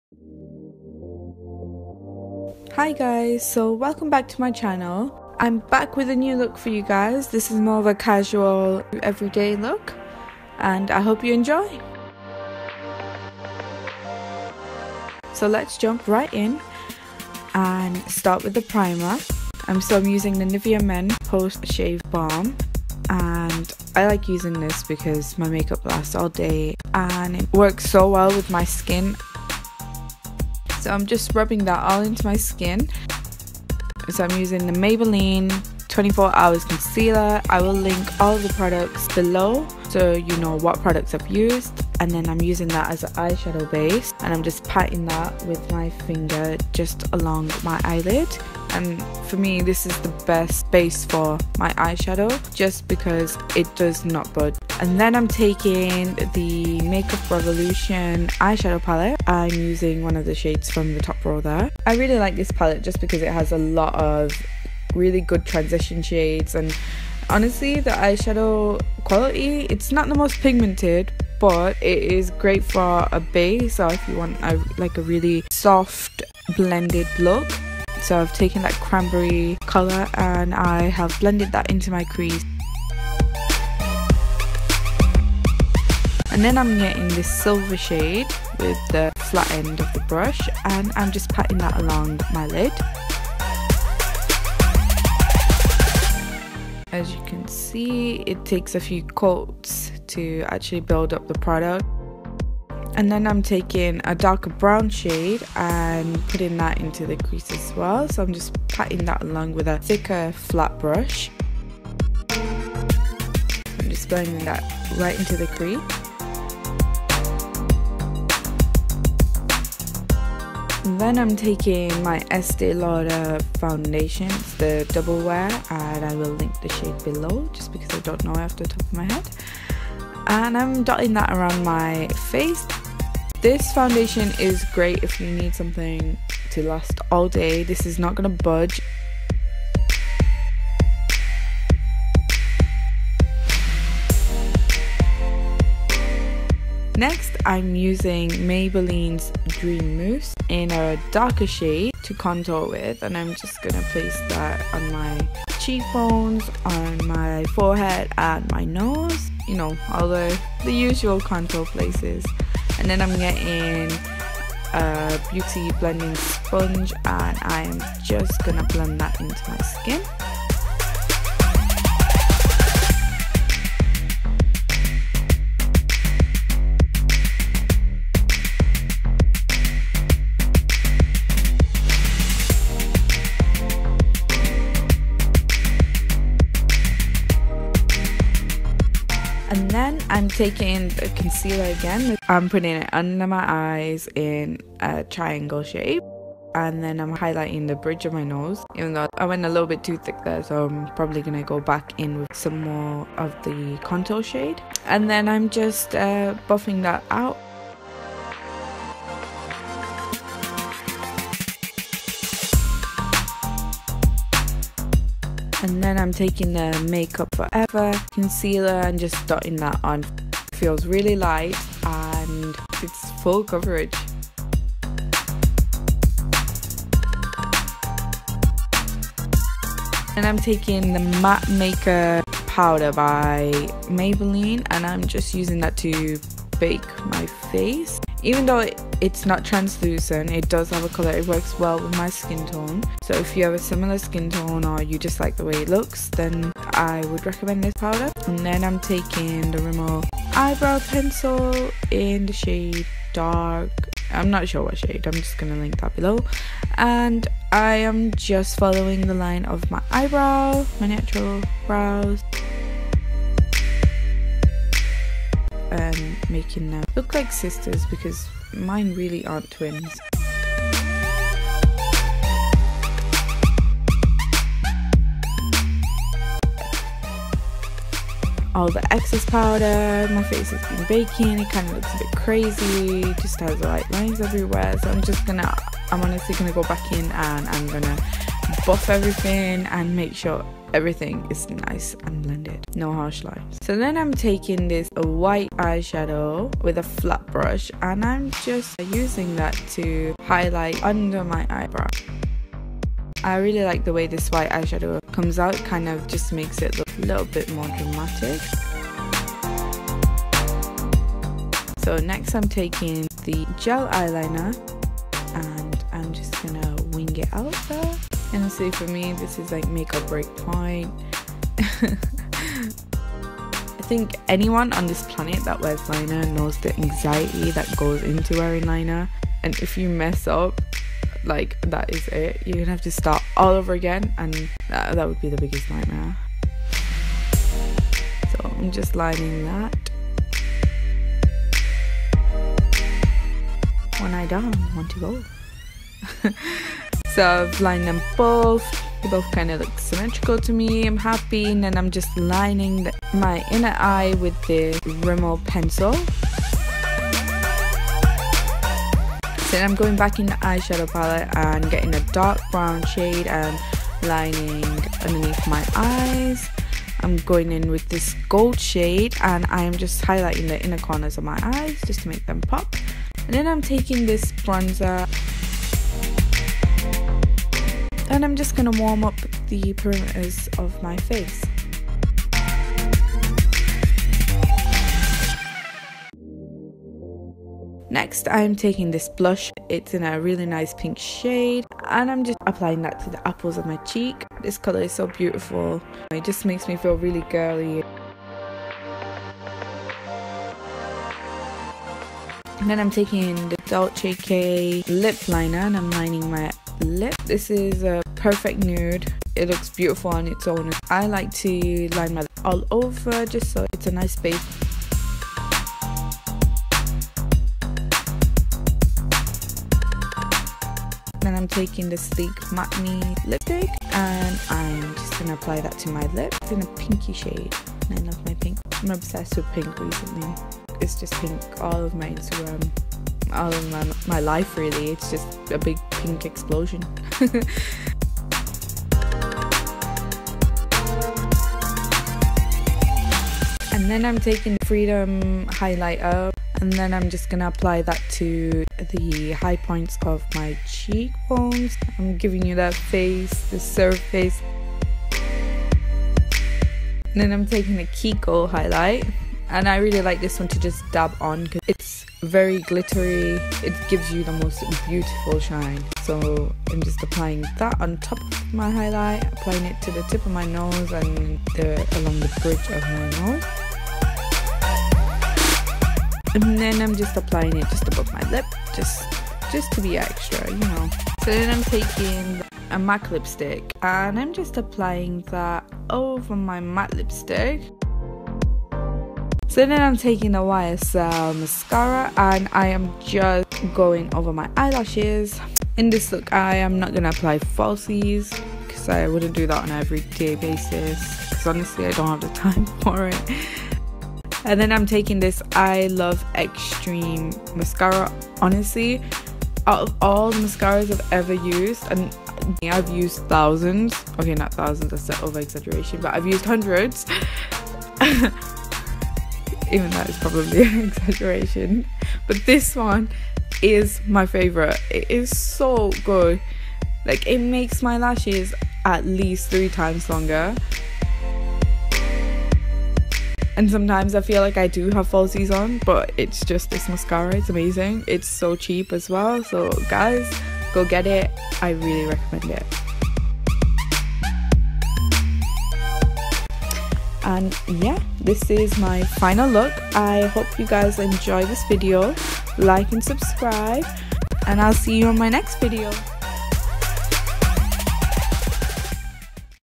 SIMPLE EVERYDAY MAKEUP
Hey guys! So here's my take on a simple everyday makeup look that you can rock at pretty much any occasion!

Nivea Men Post Shave Balm
Estee Lauder Double Wear Stay In Place Makeup - 3W1 Tawny
Makeup Forever Concealer - 8
Maybelline Superstay 24hr Concealer - Medium 03
Maybelline Matte Maker Mattifying Powder - 20 Nude Beige
Maybelline Dream Matte Mousse Foundation - Caramel
Rimmel London Professional Eye Brow Pencil - Dark Brown Black
Makeup Revolution Ultimate Eyeshadow Collection 2016
LOreal Paris Infallible Eyeshadow - Coconut Shake
Rimmel Waterproof Gel Liner
Rimmel Soft Khol Kajal Eye Pencil
SUNKISSED Bronzer
Kylie Jenner Lip Liner - Dolce K
Bourjois Little Round Pot Blusher - Lilas d'Or
Sleek Matte Me Lipstick - Shabby Chic
Freedom Pro Highlight - Ambient
Kiko Cosmetics Cream Radiance Highlighter
YSL Mascara
essence I love crazy volume mascara